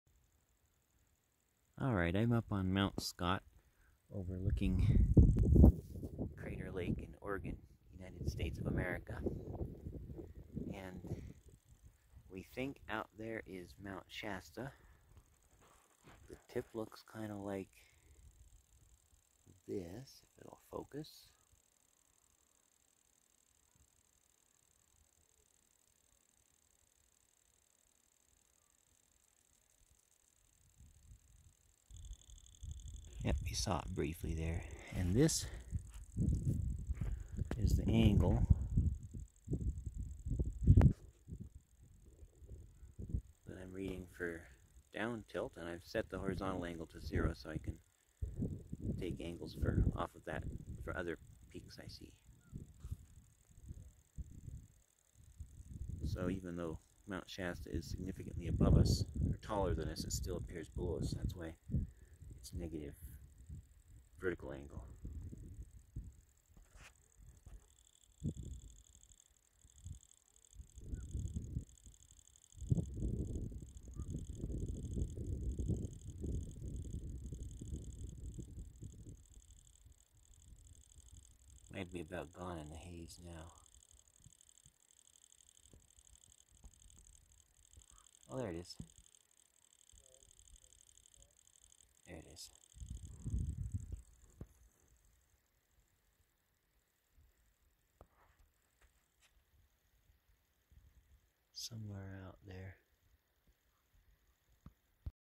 Mt. Scott to Mt. Shasta Theodolite shot 105 miles
Observer location:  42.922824,-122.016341,  8934 feet elevation
Target location:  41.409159,-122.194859,  14179 feet elevation
Distance: 105 miles.

Date and time: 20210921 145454